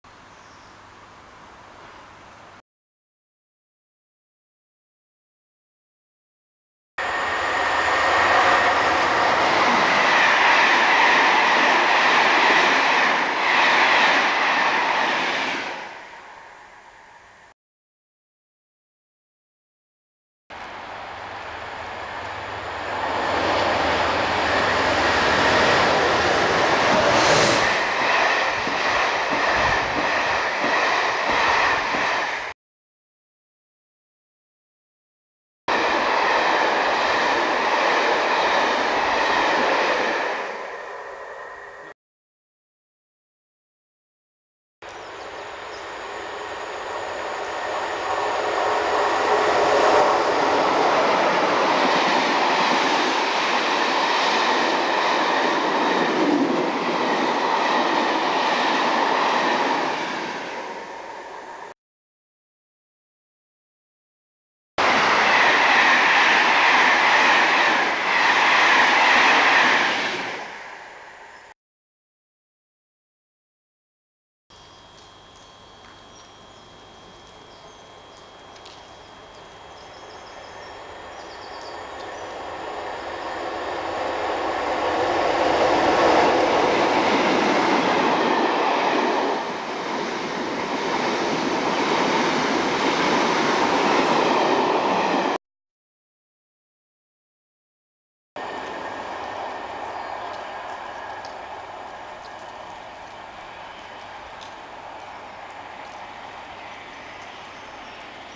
47798 with the Royal Train at Hampton in Arden 120400
47798 is seen at the end of the video bringing the Royal Train through Hampton in Arden. Before that passes through, we see: 87004, 47814, 47822, 86259 and 87008. Filmed: 12/04/2000.